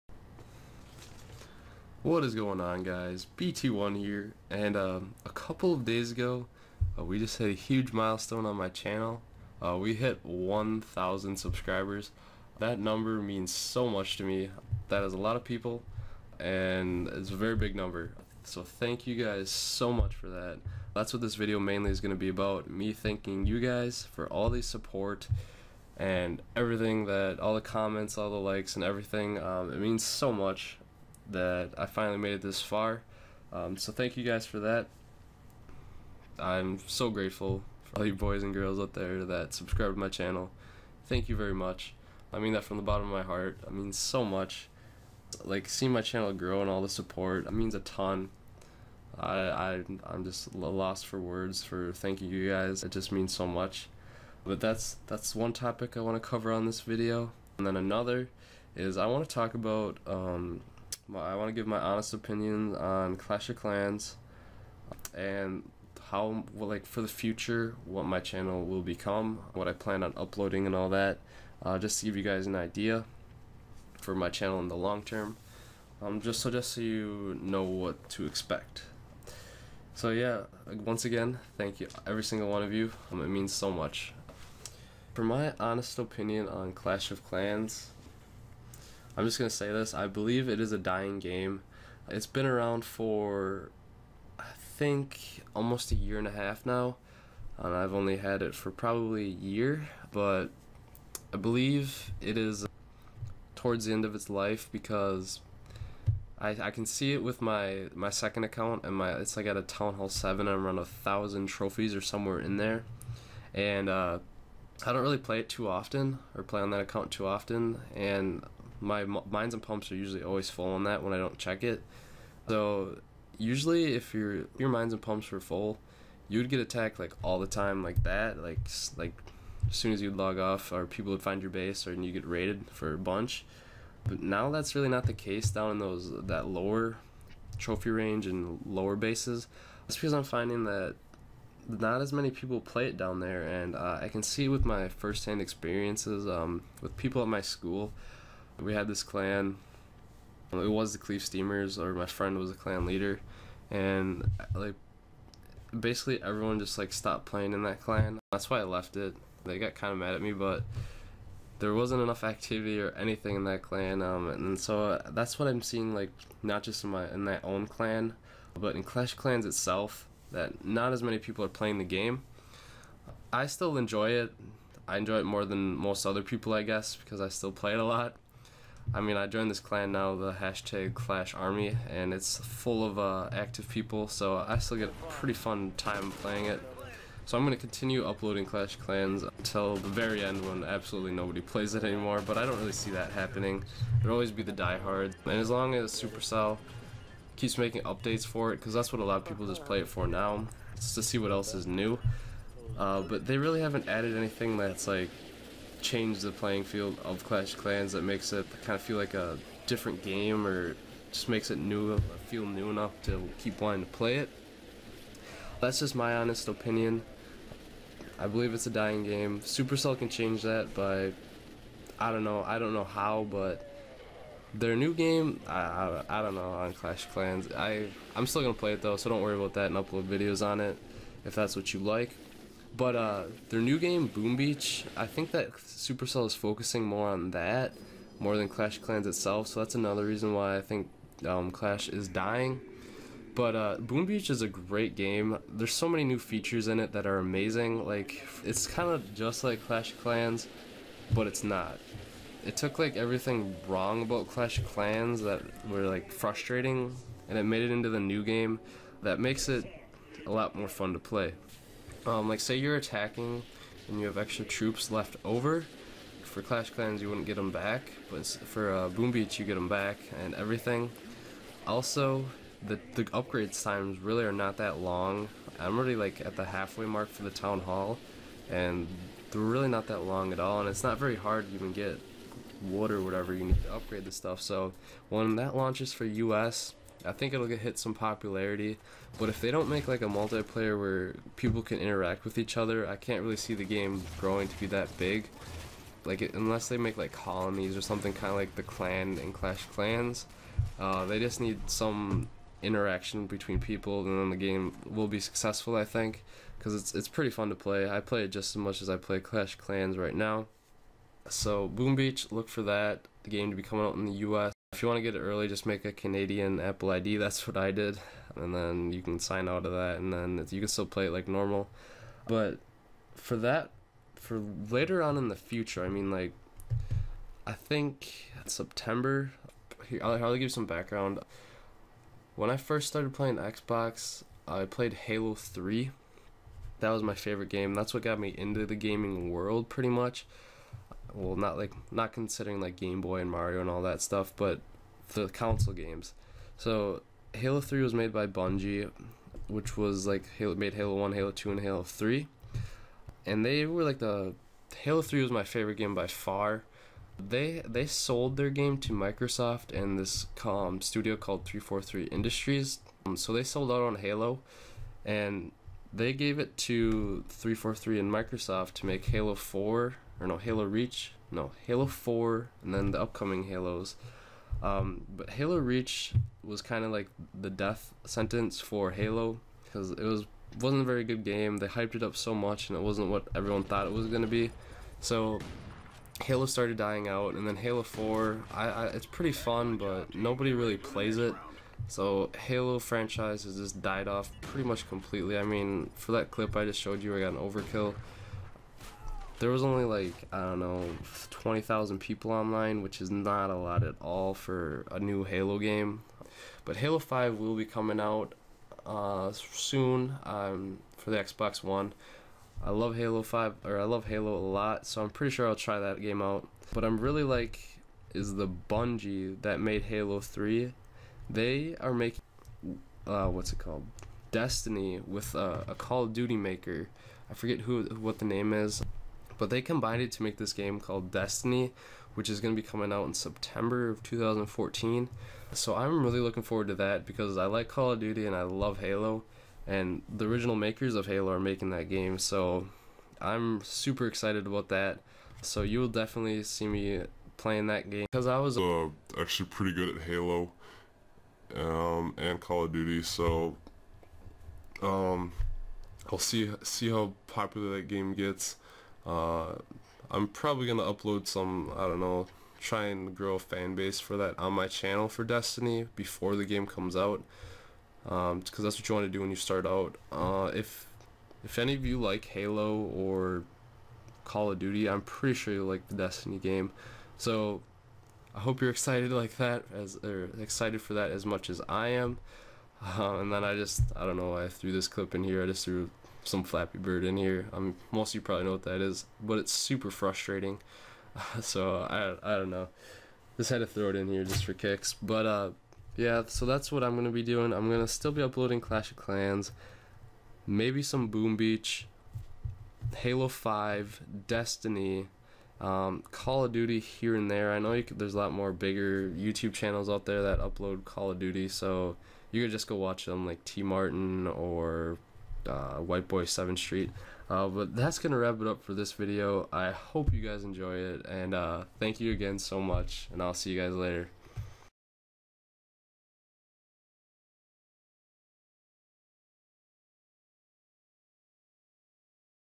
1000 Subscriber Thank You Video!
Hey guys this video is me giving you thanks for the awesome support and helping me reach 1000 subs! Also, I let you know what direction I want to take with my channel and what I plan to upload in the future!

If you know what app bounty is make an account using this
or just go to appbounty,net and join using my invite code "bt1"